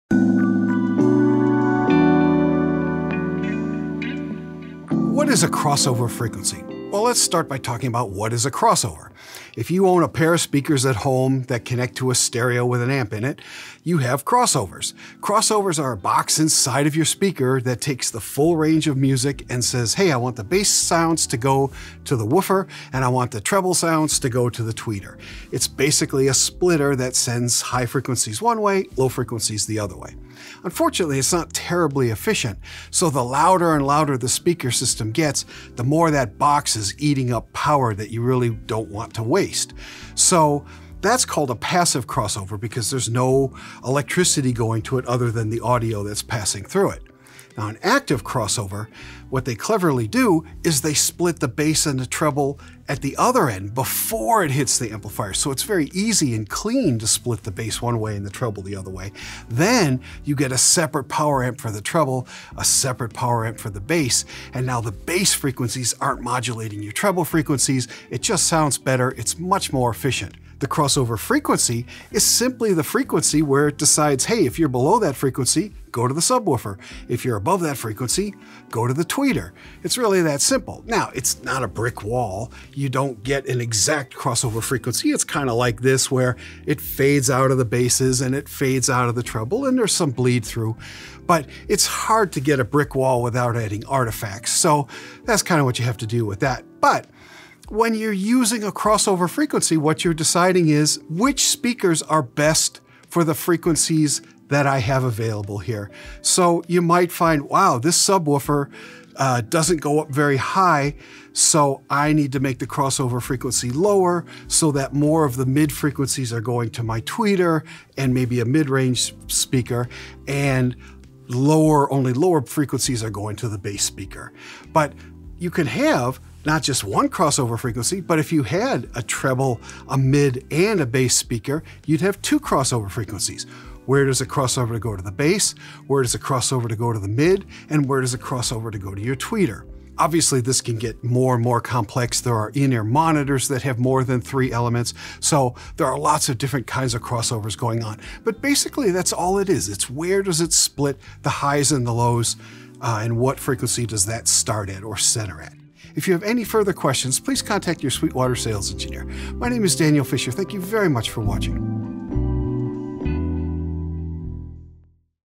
What Is Crossover Frequency? – Daniel Fisher
If you‘re wondering what video gear we use, check it out

Daniel Fisher, Sweetwater‘s resident synth expert, goes in depth on everything related to crossover frequency in speakers, headphones, studio monitors, and more.

After you watch, check out Sweetwater.com today for all your music instrument and pro audio needs!

0:00 – Intro and Crossover Definitions
0:14 – Passive Crossovers
0:30 – Active Crossovers
1:10 – Crossover Frequency
1:37 – Choosing Crossover Frequency
2:05 – Multiple Crossover Frequencies
2:28 – Crossover Wrap Up
2:35 – Thanks for Watching!